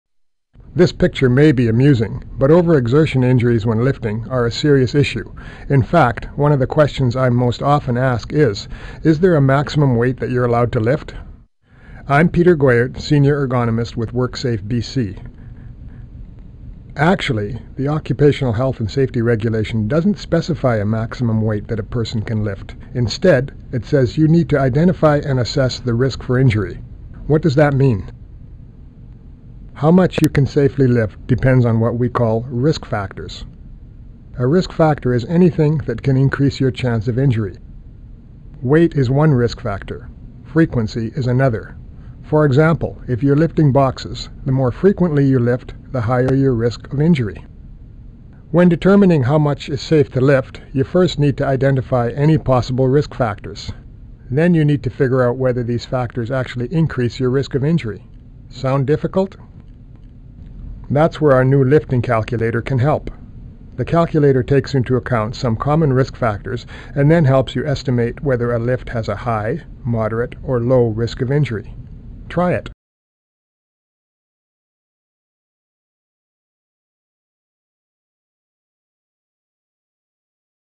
Lifting Calculator
Ever wondered whether a weight was safe to lift? Our new lifting calculator helps estimate whether a lift has a low, moderate, or high risk of injury.